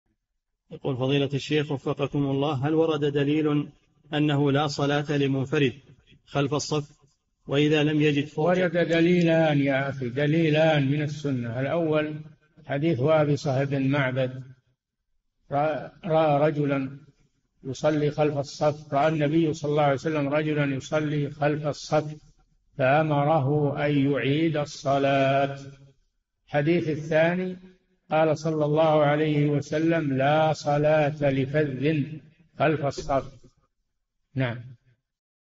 [3221- 4717] هل ورد دليل على أنه لا صلاة لمنفرد خلف الصف؟ - الشيخ صالح الفوزان
"[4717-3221] هل ورد دليل أنّه لا صلاة لمنفرد خلف الصف وإذا لم يجد؟
الجَوَابُ: ورد دليلان يا أخي، دليلان من السُّنة: الأول: حديث وابصة بن معبد «رَأَى النبي ﷺ رَجُلًا يُصَلِّي خَلْفَ الصَّفِّ فأمره أن يُعيدَ الصَّلاةَ»، الحديث الثاني: قال ﷺ: « لَا صَلَاةَ لِفَذٍ خَلْفَ الصَّفِّ ».

للاشتراك في قناة الشيخ صالح الفوزان
فتاوى المجالس في تفسير المفصل

للاستفسار والإقتراحات والدعم
رئيس الفريق
لمشروع كبار العلماء